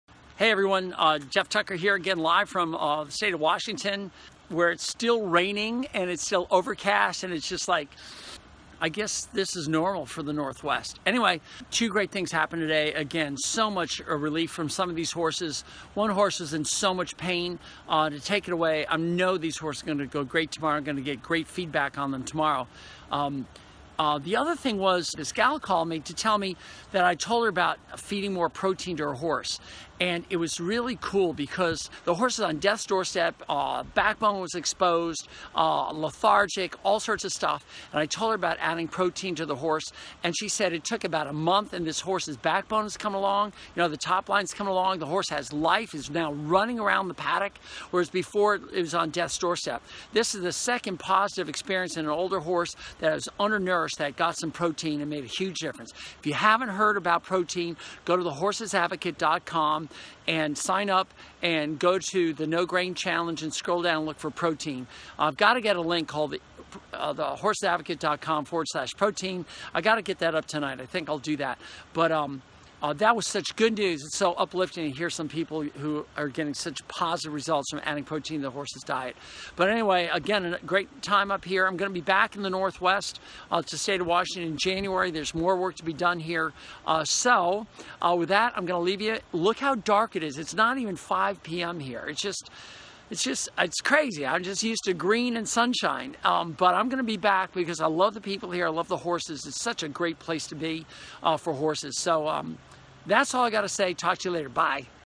Protein diet success story
Today, a gal called me to tell me that I told her about feeding more protein to her horse. Now, this horse was practically on death's doorstep, and after only a month of adding protein to this horse's diet - the results are in.